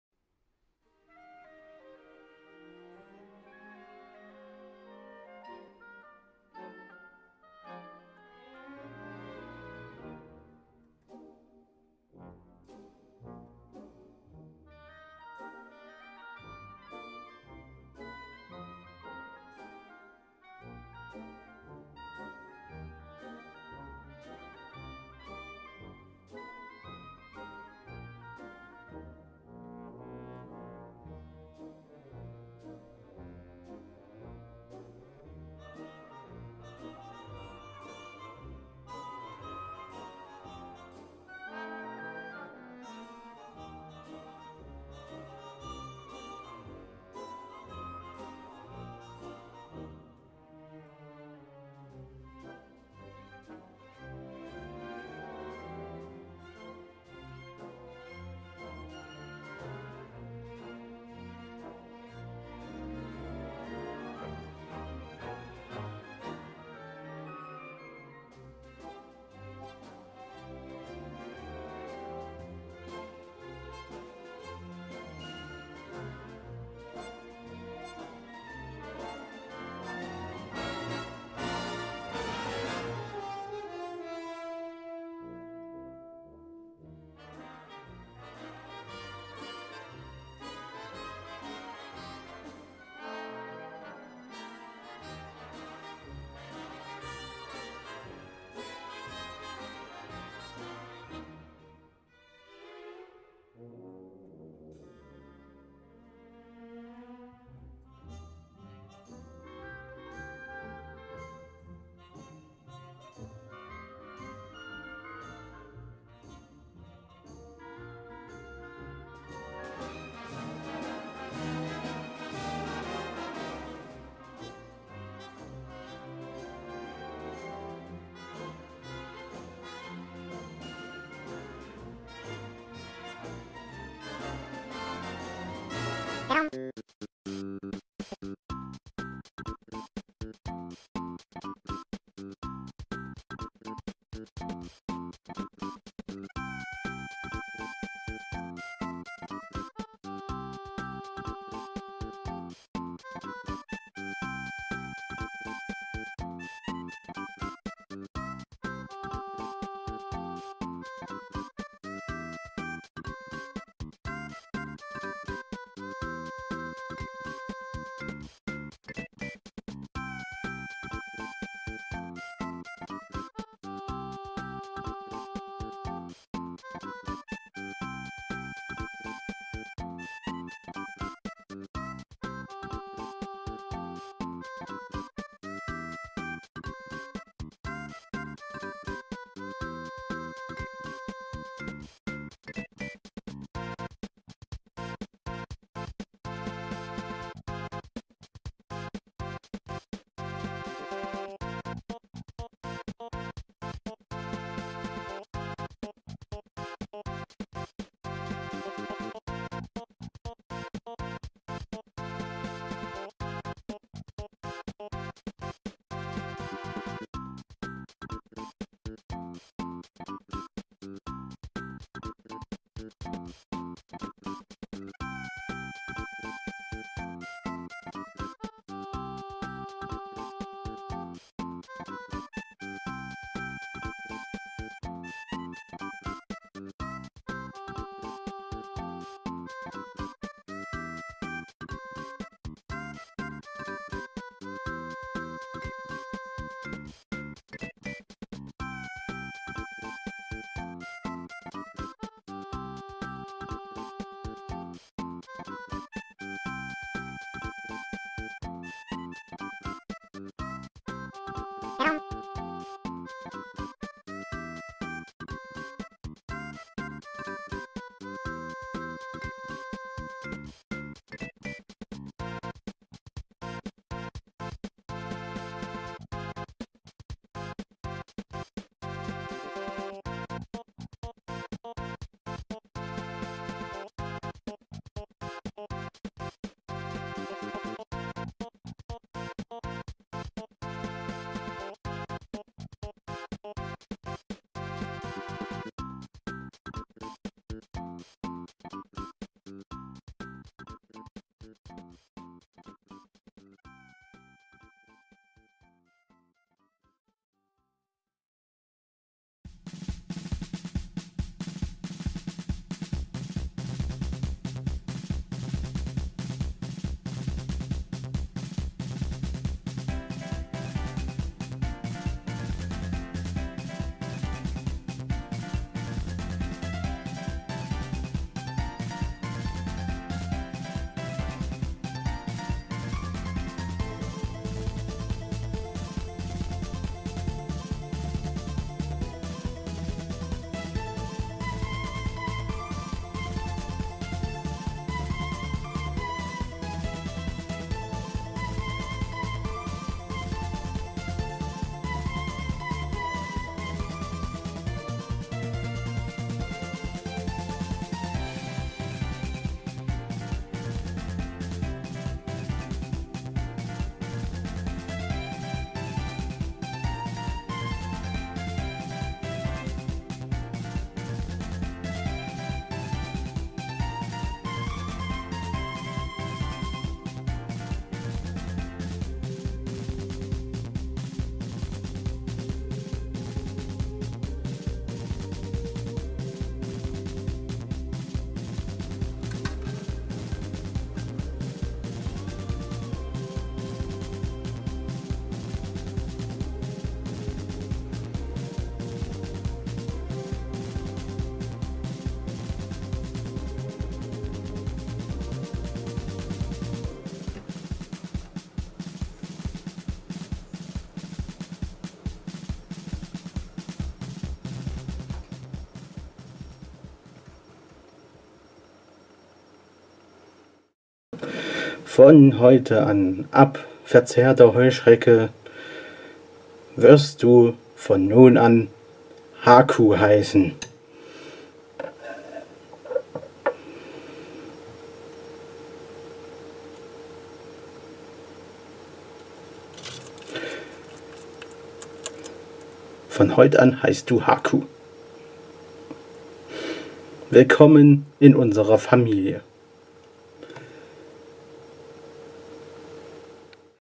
Haku die Neue Generation!
Heißt Haku in unsere Famile Willkommen ! Eine Neue Bartagame ist eingezogen !